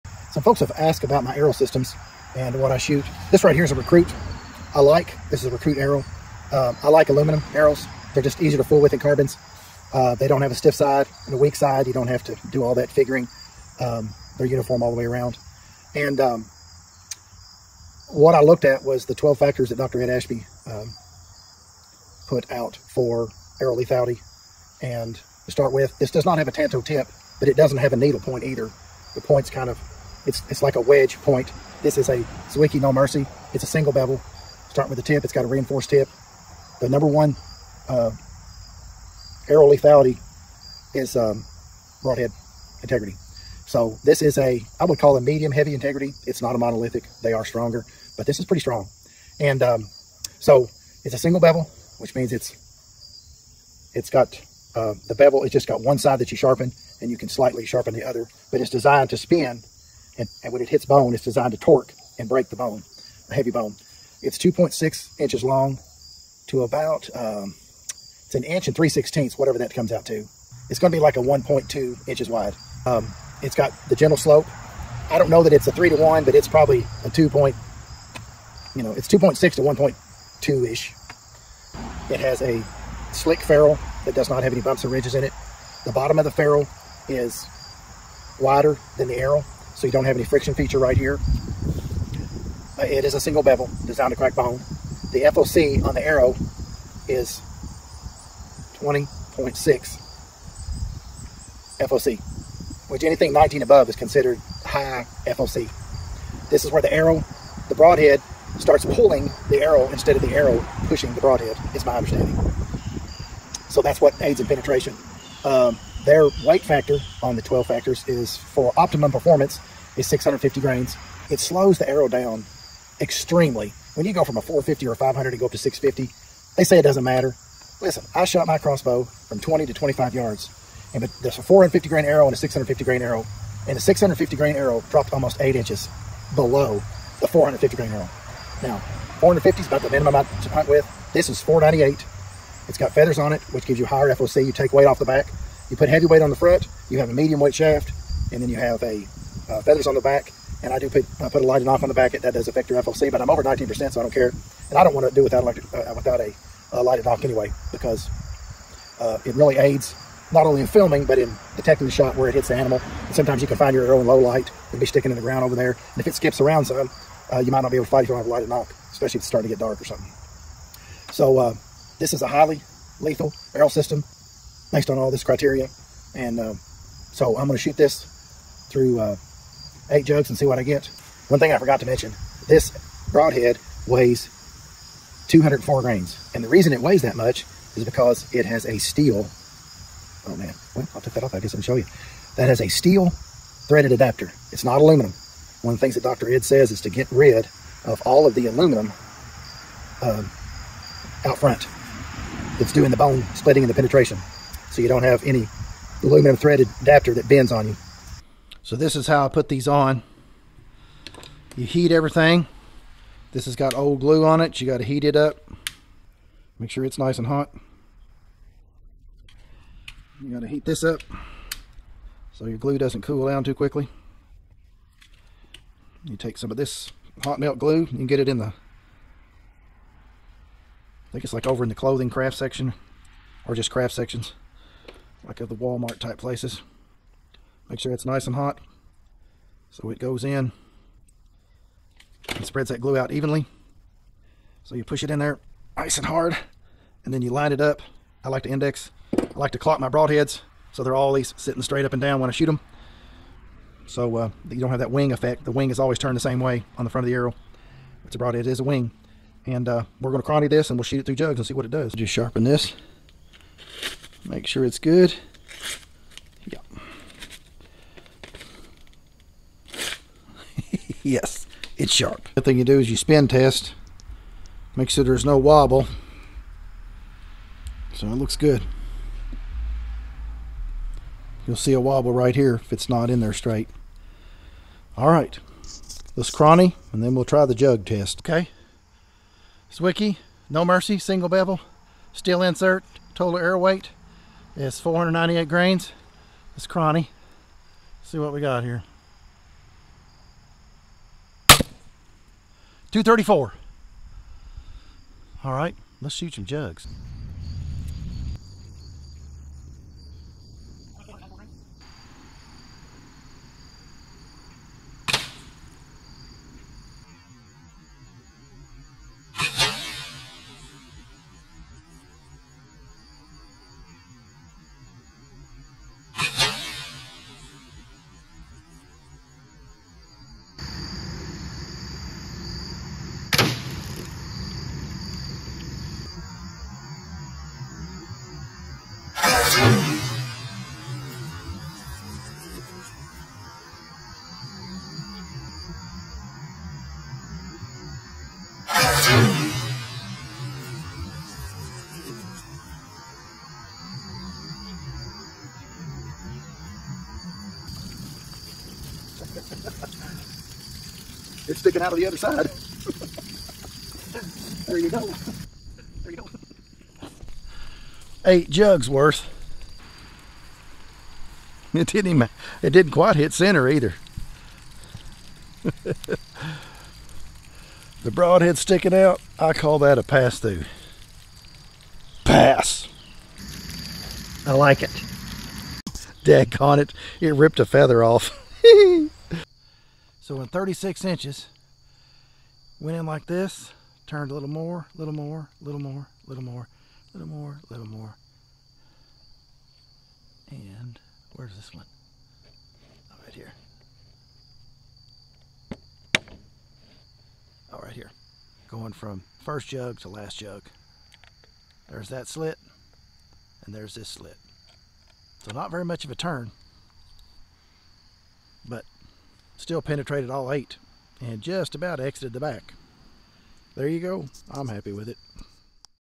Barnett Wildcat CRX Recruit Zwickey No Mercy Eight 2 liters
Arrow system explanation. This is just a sample of many arrow setups I shoot. I experiment with all types of arrows, arrow weights, point weights and different types of broadheads. If you want to know more about the science behind hunting arrow lethality, look up information regarding Dr. Ed Ashby's African game harvest study that was used to determine what arrow characteristics could survive heavy bone impacts and penetrate sufficiently to dispatch heavy boned large game animals. Troy Fowler's Youtube channel "Ranch Fairy" also has a ton of information on arrow lethality. Note: The Wildcat CRX bow is on the shorter Recruit Crossbow stock. This arrow system will shoot about 20 fps faster on the CRX bow on the CRX stock due to a longer power stroke which pulls the bow back one inch further.